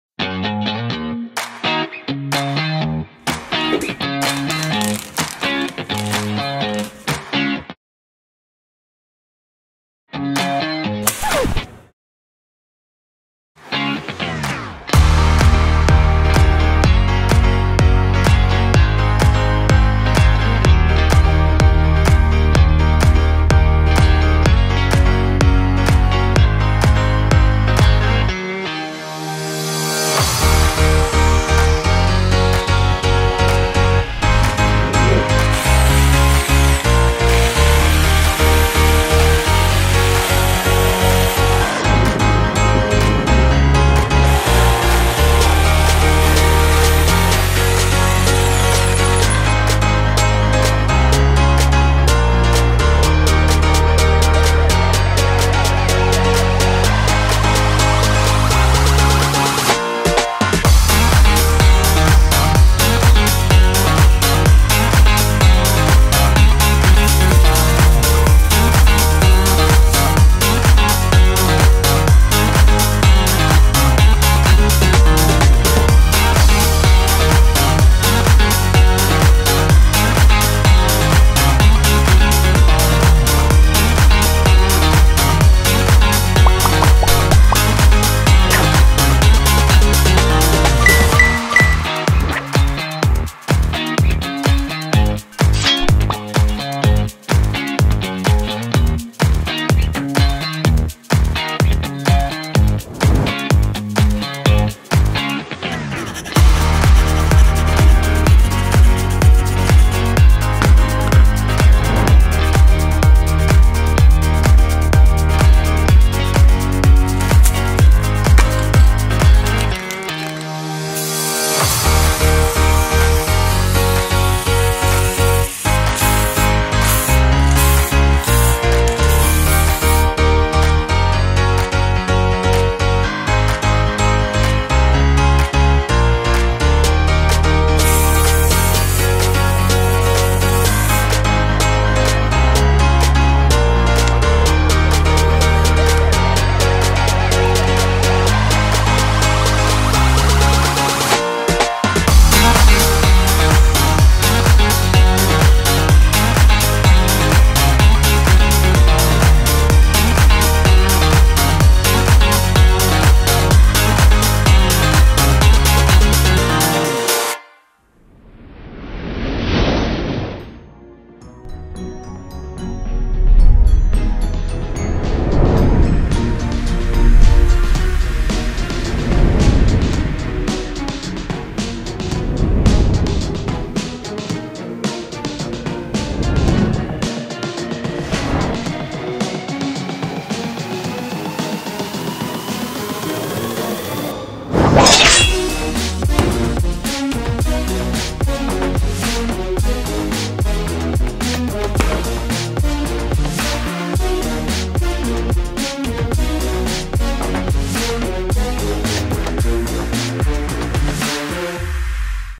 Cool Cyberpunk Katana Out Of Popsicle Stick
Rather than throwing it away, it's better to turn it into a katana.